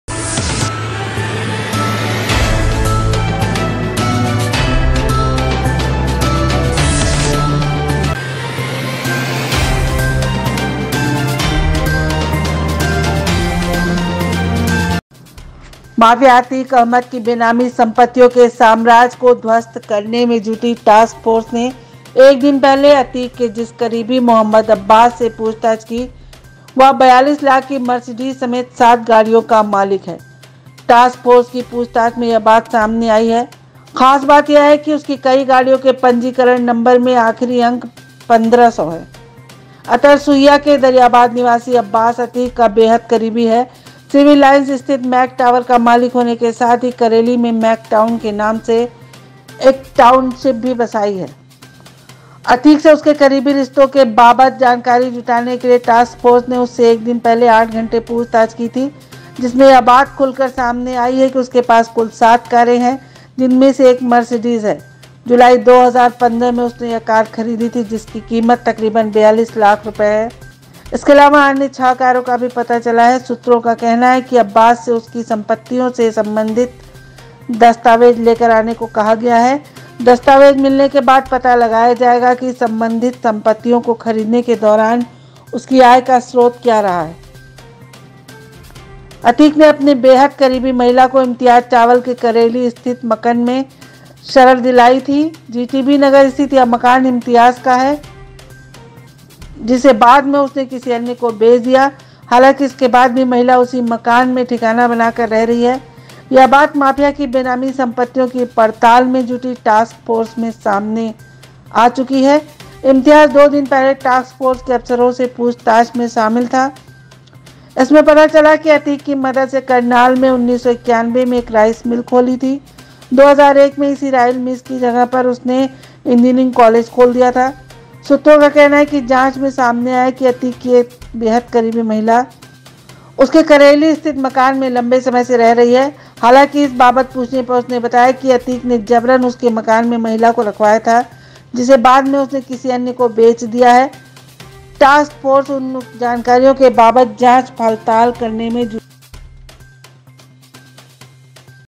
atiqueahmed मर्सिडीज समेत सात गाड़ियों का मालिक है अब्बास,पूछताछ में खुलासा
अतीक की बेनामी संपत्ति : 42 लाख की मर्सिडीज समेत सात गाड़ियों का मालिक है अब्बास, पूछताछ में खुलासा

माफिया की बेनामी संपत्तियों के साम्राज्य को ध्वस्त करने में जुटी टास्क फोर्स ने एक दिन पहले अतीक के जिस करीबी मो.अब्बास से पूछताछ की, वह 42 लाख की मर्सिडीज समेत सात गाड़ियों का मालिक है। टास्क फोर्स की पूछताछ में यह बात सामने आई है। एक खास बात यह भी है कि उसकी कई गाड़ियों के पंजीकरण नंबर में आखिरी चार अंक 1500 हैं। अतरसुइया के दरियाबाद निवासी अब्बास अतीक का बेहद करीबी है। सिविल लाइंस स्थित मैक टावर का मालिक होने के साथ ही उसने करेली में मैक टाउन नाम से एक टाउनशिप भी बसाई थी। अतीक से उसके करीबी रिश्तों के बाबत जानकारी जुटाने के लिए ही टास्क फोर्स ने उससे एक दिन पहले आठ घंटे तक पूछताछ की थी। पूछताछ में यह बात सामने आई कि उसके पास कुल सात कार हैं। इनमें से एक मर्सिडीज भी है। जुलाई 2015 में यह कार उसने खरीदी थी, जिसकी कीमत करीब 42 लाख रुपये थी। इसके अलावा छह अन्य कारों के बारे में भी पता चला है।

पुलिस सूत्रों का कहना है कि अब्बास से उसकी संपत्तियों से संबंधित दस्तावेज लेकर आने को कहा गया है। दस्तावेजों के मिलने के बाद पता लगाया जाएगा कि संबंधित संपत्तियों को खरीदने के दौरान उसकी आय का क्या स्रोत रहा। अतीक ने अपनी एक बेहद करीबी महिला को इम्तियाज चावल के करेली स्थित मकन में शरण दिलाई थी। जीटीबी नगर स्थित यह मकान इम्तियाज का ही था, जिसे बाद में उसने किसी अन्य को बेच दिया। हालांकि, इसके बाद भी महिला उसी मकान में ठिकाना बनाए रही। यह बात माफिया की बेनामी संपत्तियों की पड़ताल में जुटी टास्क फोर्स की जांच में सामने आई है।

इम्तियाज से दो दिन पहले टास्क फोर्स के अफसरों ने पूछताछ की थी। इसमें पता चला था कि उसने अतीक की मदद से करनाल में 1991 में एक राइस मिल खोली थी। 2001 में इसी राइस मिल की जगह पर उसने इंजीनियरिंग कॉलेज खोल दिया था। पुलिस सूत्रों का कहना है कि जांच में सामने आया कि अतीक की एक बेहद करीबी महिला उसके करेली स्थित मकान में लंबे समय से रह रही है। हालांकि, इस बाबत पूछने पर उसने बताया कि अतीक ने जबरन उसके मकान में महिला को रखवाया था। जिसे बाद में उसने किसी अन्य व्यक्ति को बेच दिया। टास्क फोर्स उक्त जानकारियों के बाबत जांच पड़ताल में जुटी है।